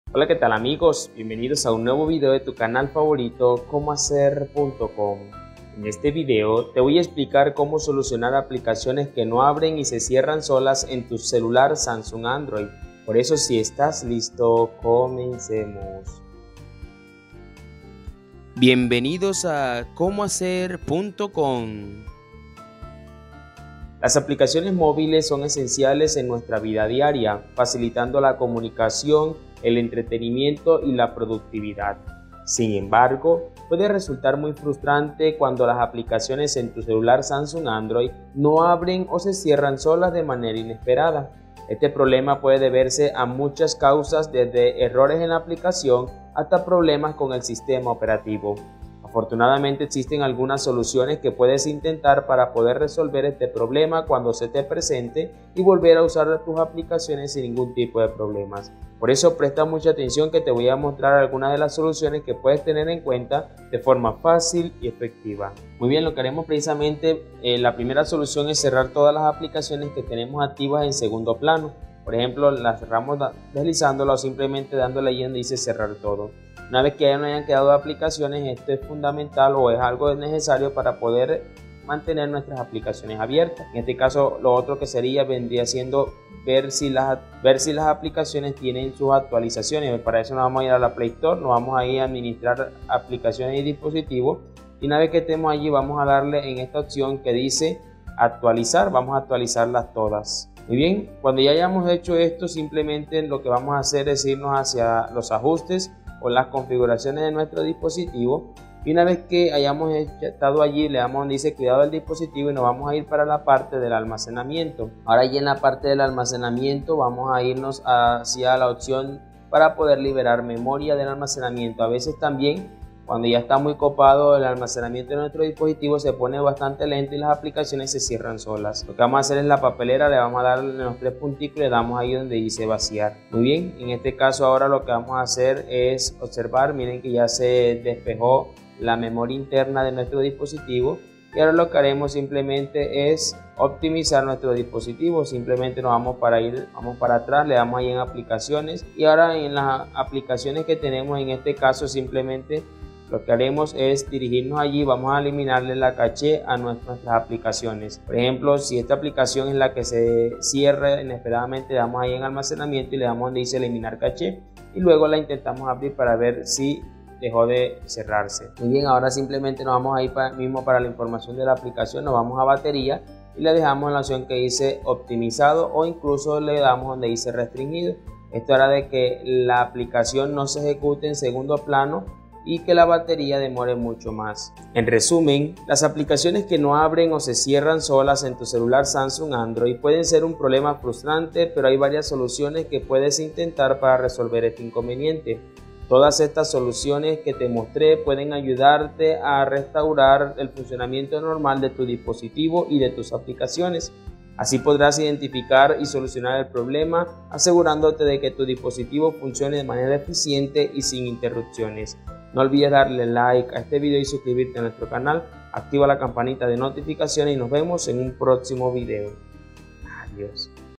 📱 Cómo Solucionar APLICACIONES QUE NO ABREN Y SE CIERRAN SOLAS en mi Celular Samsung Android 📱
En este video profundizaremos en las causas comunes y soluciones fáciles para lidiar con un problema que todos tememos enfrentar: aplicaciones que se niegan a abrir o que se cierran de repente en tu celular Samsung Android. No te dejes dominar por la frustración; te guiaremos paso a paso a través de varias estrategias de solución, desde los métodos más simples hasta técnicas más avanzadas.